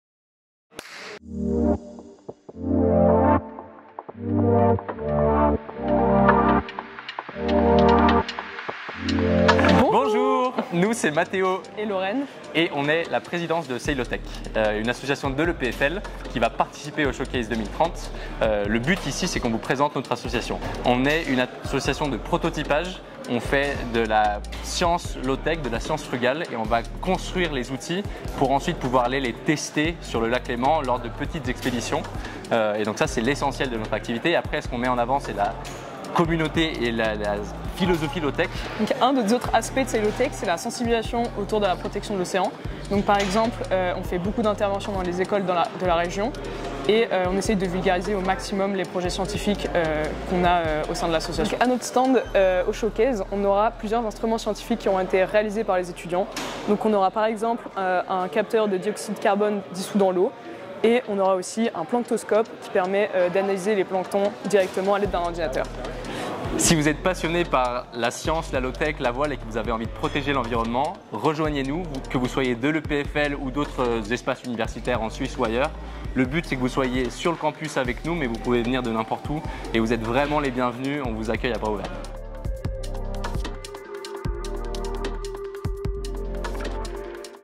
Showcase 2030: Meet our Exhibitors - Sailowtech
As one of 30 inspiring exhibitors of the 2024 edition of Showcase 2030, Sailowtech’s mission focuses on promoting frugal, low-tech research to protect marine ecosystems, combining environmental preservation with scientific exploration. They aim to raise awareness about the critical role oceans play in mitigating climate change.

Meet them and other insipiring solutions at Showcase 2030 on the 29th of October.